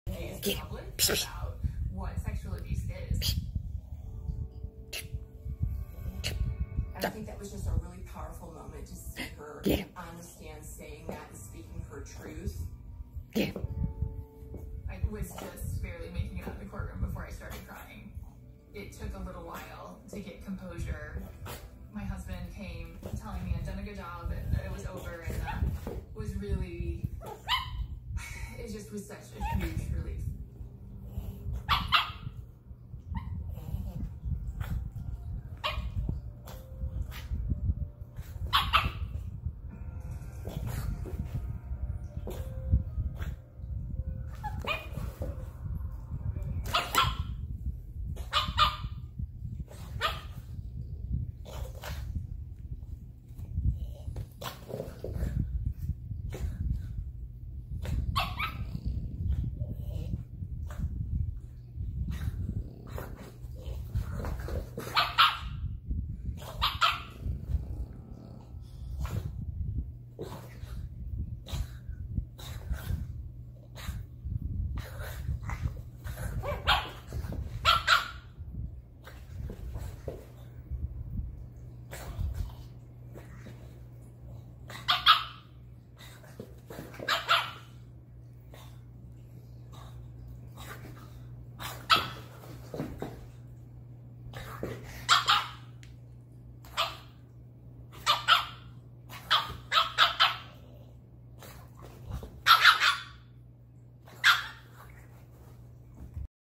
Cute pup video of the day
New Chihuahua pup getting to know her big Frenchie sister.